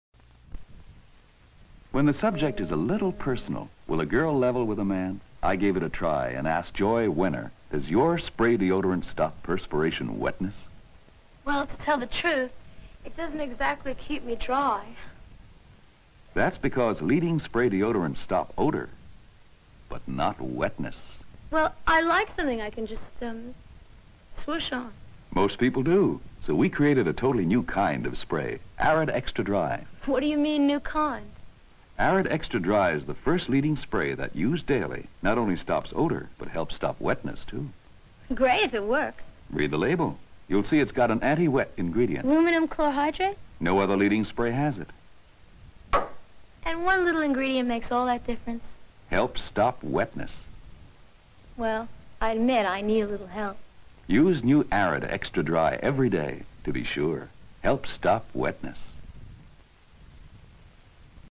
1972 Commercial for Arrid Extra Dry with Joy Wener (Joy Bang)
Lady is concerned about wetness (oh don't ask). Narrator gleefully reccomends product with Aluminum Chlorjydrate. Lady is SOLD.